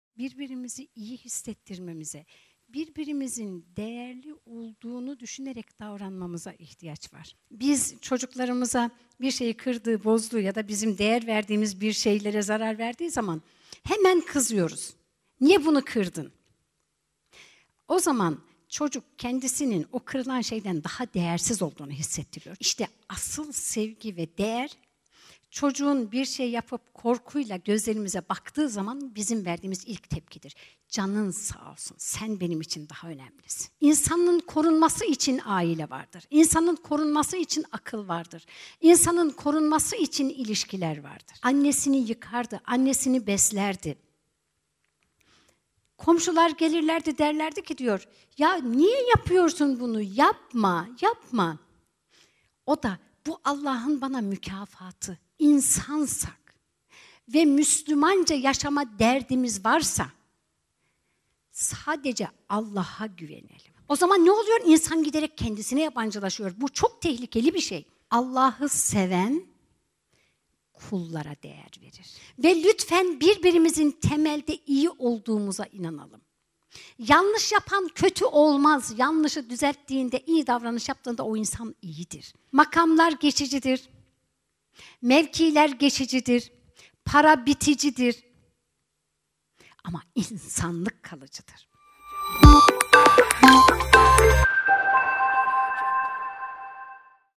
Kısa Kısa | Saliha Erdim ile Huzurlu Aile Sohbetleri - "İnsanlık Kalıcıdır"
Hayat Mektebi'nin tüm bölümlerini izlemek için Esenler Belediyesi Hoca Ahmet Yesevi Kültür Sanat Sezonu kapsamında düzenlenen "Huzurlu Aile Sohbetleri" programının tamamını izlemek için linke tıklayınız!